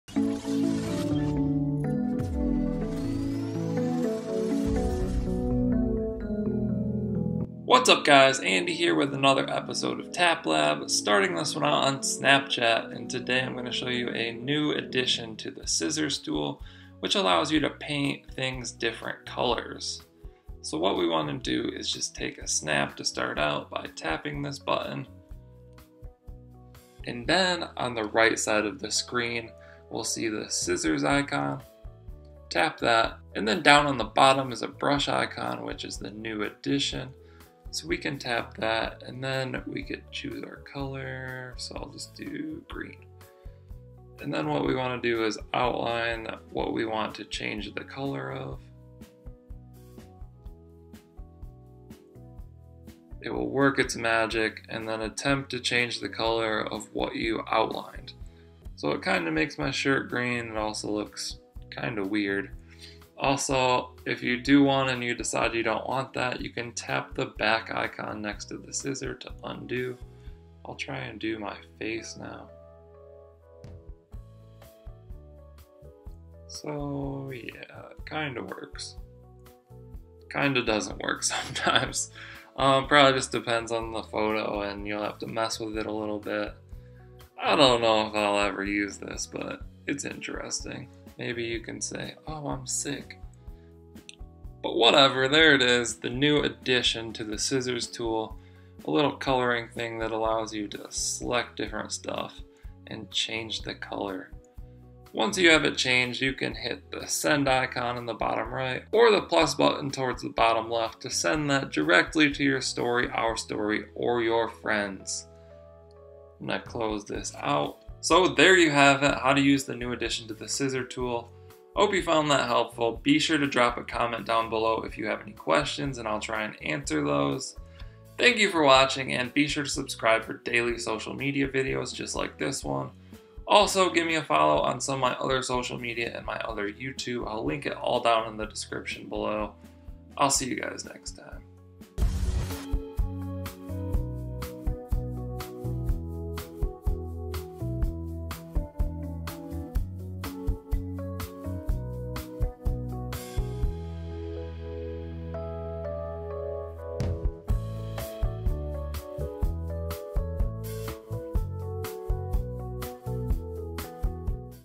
How to Use Snapchat Scissor Brush Tool - Update 10.13.0.0
How to Use Snapchat Scissor Brush Tool - Update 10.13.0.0 - In this video I give a tutorial on how to use the new snapchat brush tool within the scissor button. You can outline things and change the color of them.
TapLab is dedicated to making the best videos about tips/tricks, reviews, experiments, and more with mobile apps and devices.